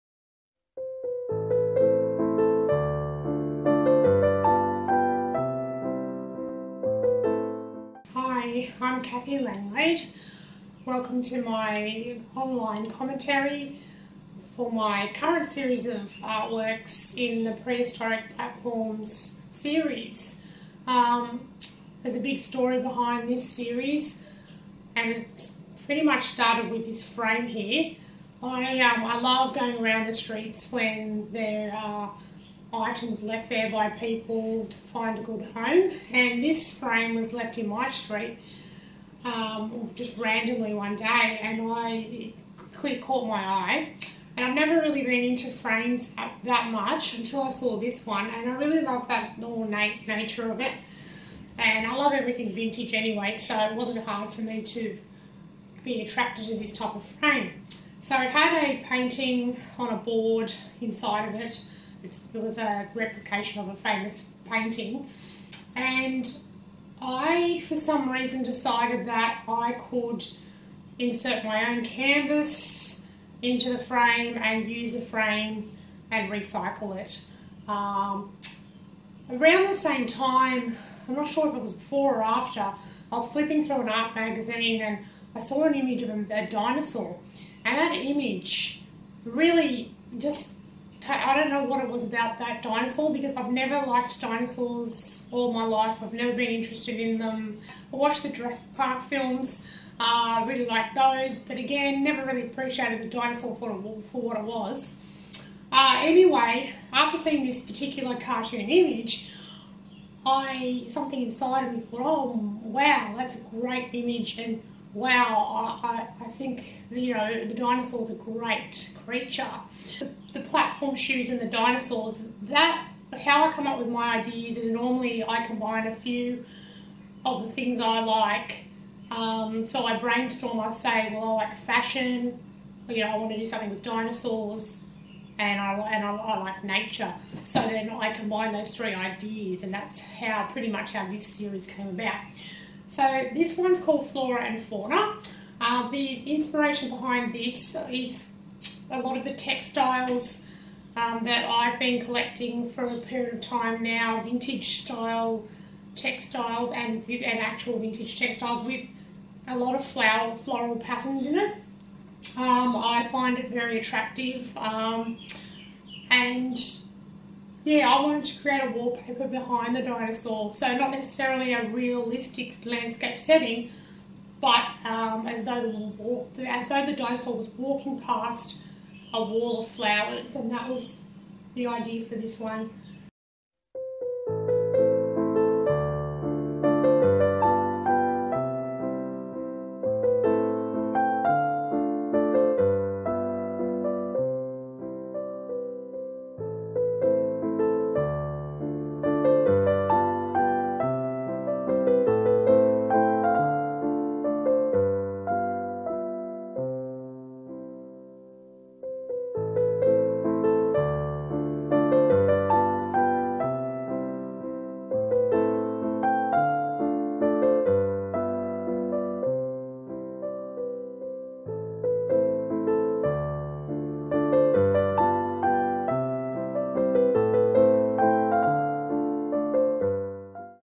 Flora and Fauna
This video introduces you to my new series of artworks called Prehistoric Platforms, featuring paintings of dinosaurs wearing platform shoes. The first video discusses ideas and inspiration behind the work called Flora and Fauna with progress photos of the artwork at the end of the video.